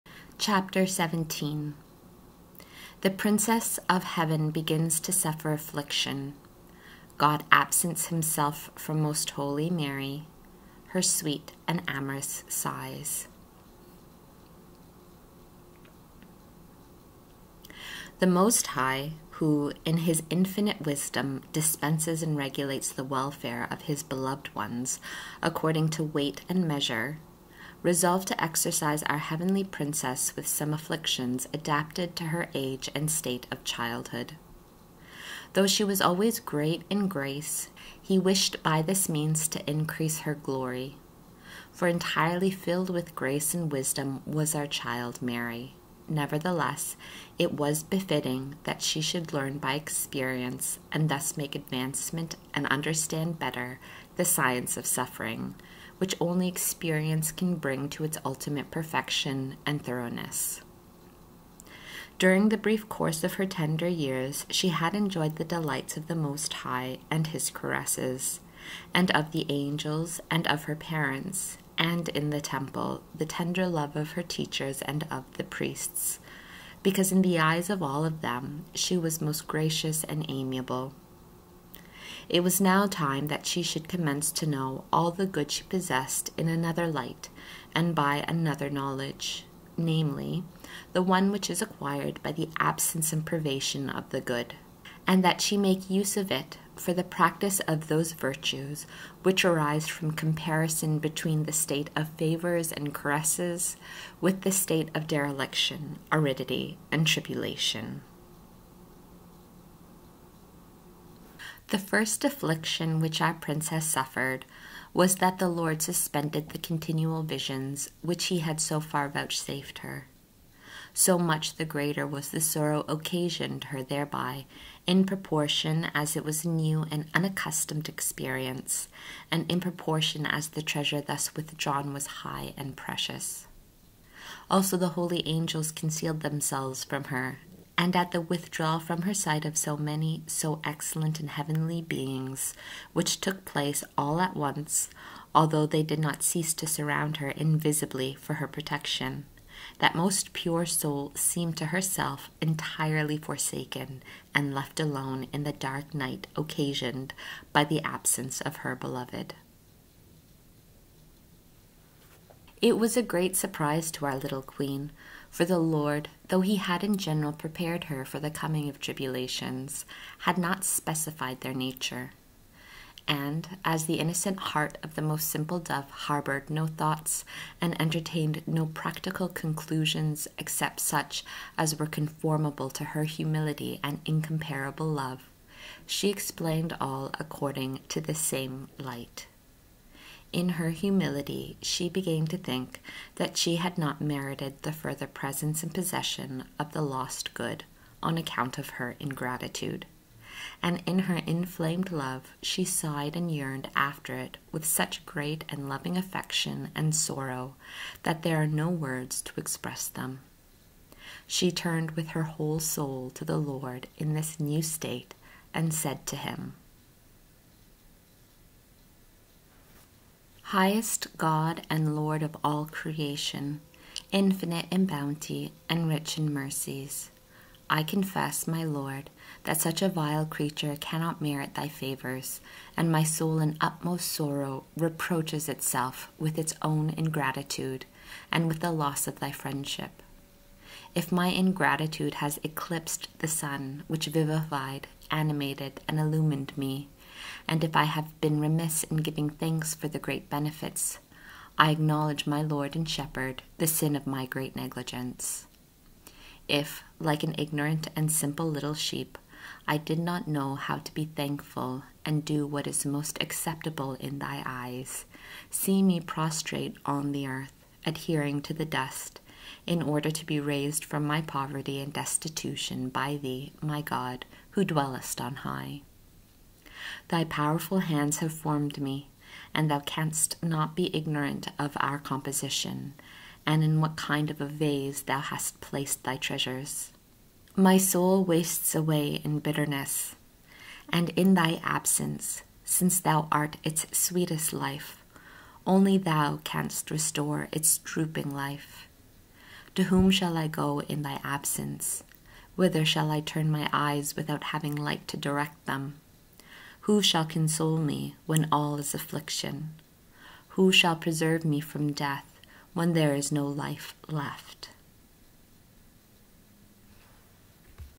Mystical City of God - Book 2 - Chapter 17 (Part 1/3)
Image - The Guardian Angel by Sir Edward Coley Burne-Jones

Paragraphs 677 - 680 of Book 2 of the Mystical City of God by Mary of Jesus of Ágreda.